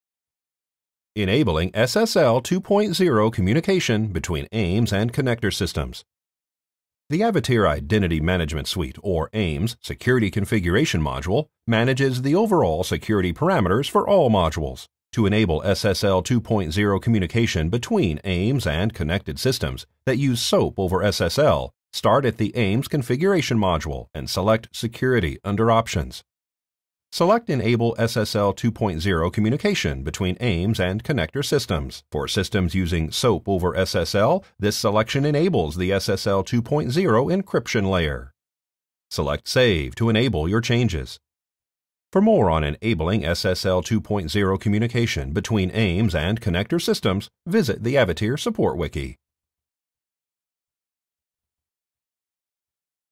Enabling SSL 2.0 Communication Between AIMS and Connector Systems
The Avatier Identity Management Suite, or AIMS Security Configuration module manages the overall security parameters for all modules.

To enable SSL 2.0 communication between AIMS and connected systems, that use SOAP over SSL start at the AIMS configuration module. And, select Security under Options. Select Enable SSL 2.0 communication between AIMS and connector systems. For systems using SOAP over SSL, this selection enables the SSL 2.0 Encryption Layer. Select Save to enable your changes. For more on enabling SSL 2.0 communication between AIMS and connector systems, visit the Avatier Support Wiki.